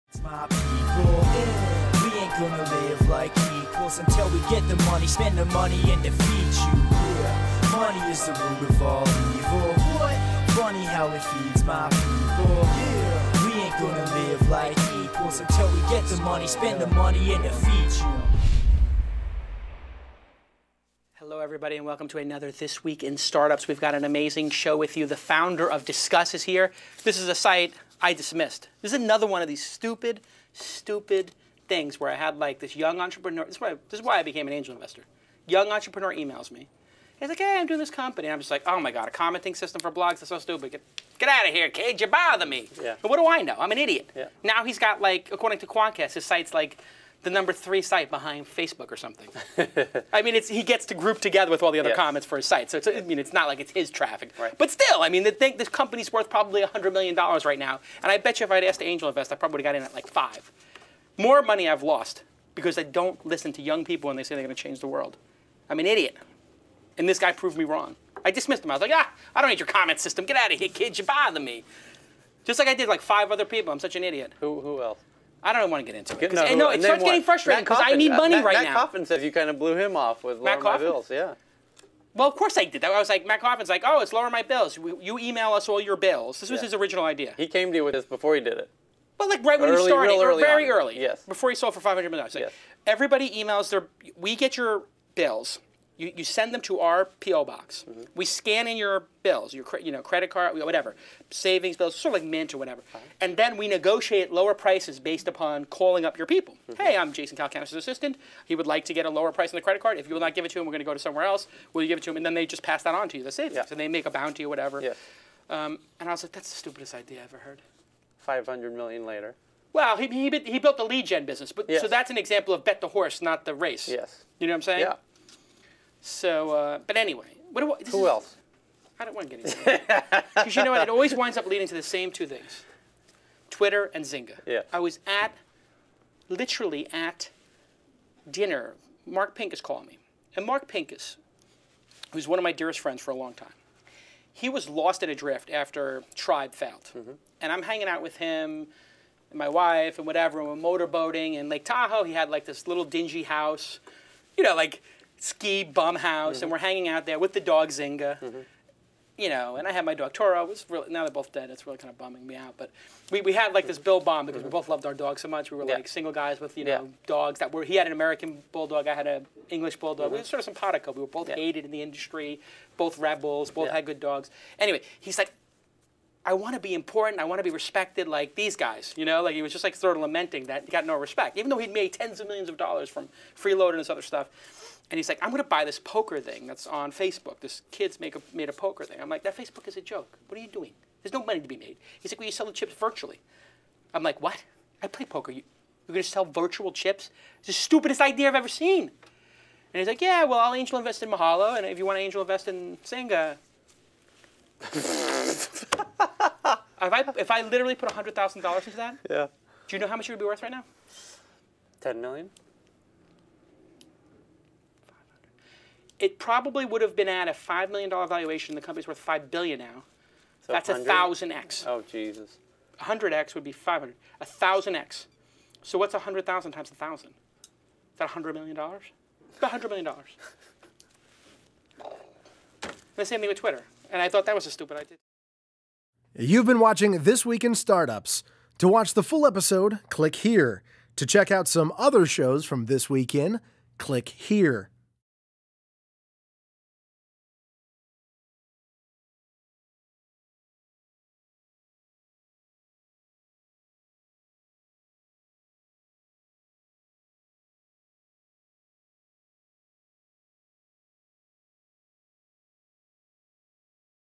Jason Calacanis loses millions...kinda
Jason recalls missed opportunities and calculates the 100+ Million he lost from not investing in soon-to-be great companies.